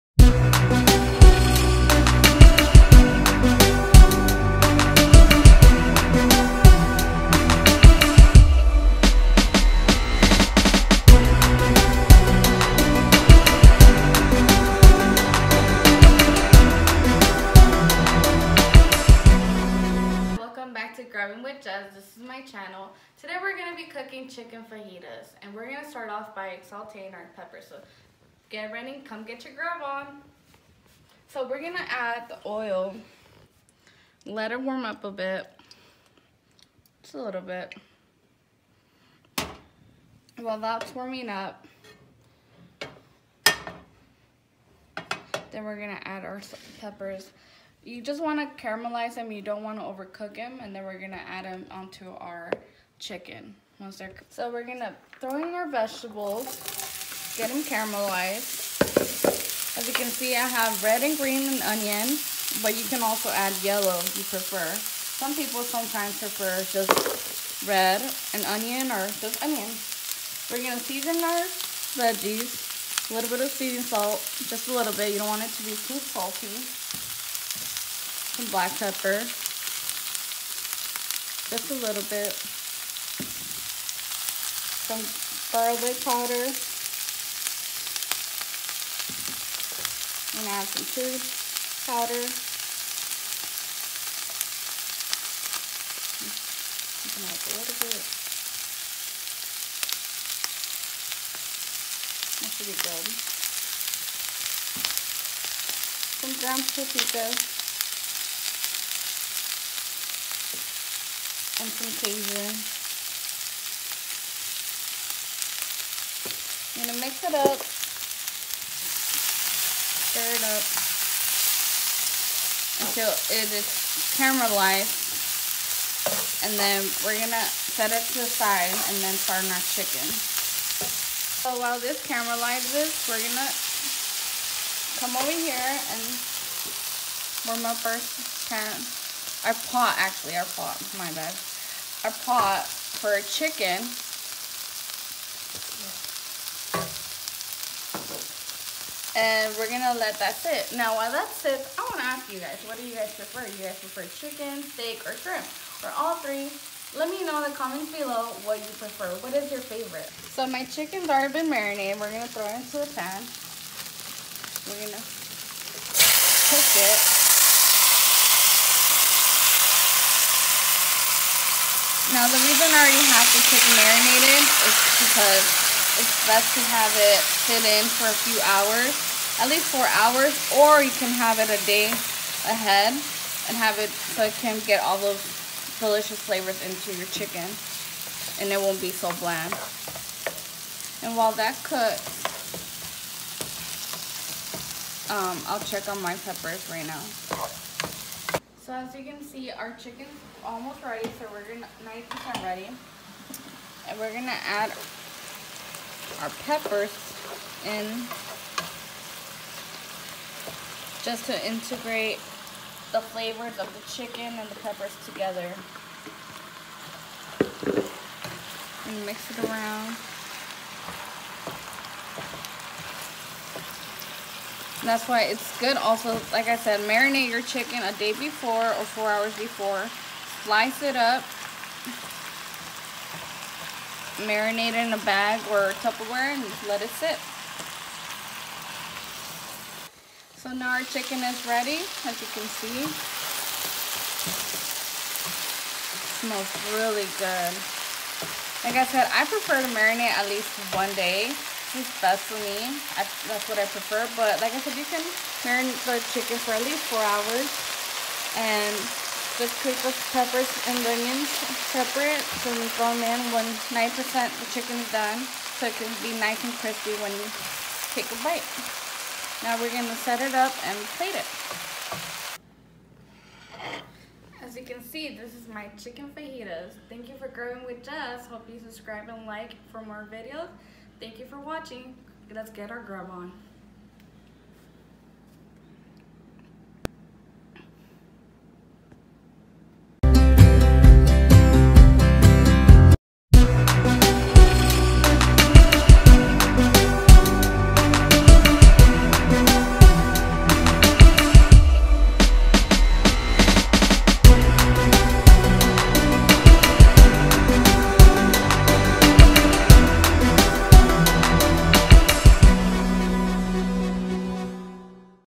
Grubbin with Jess (Chicken Fajitas)
Ingredients:
1lb Chicken breast
1 red bell pepper
1 green bell pepper
1 half onion
1 tbsp n’orleans cajun
1 tsp chili powder
1 tsp ground paprika
1 tsp oregano
1/4 tsp black pepper
1/4 tsp garlic powder
1/4 tsp onion powder
1/4 cumin ground
1/2 tbsp crushed red pepper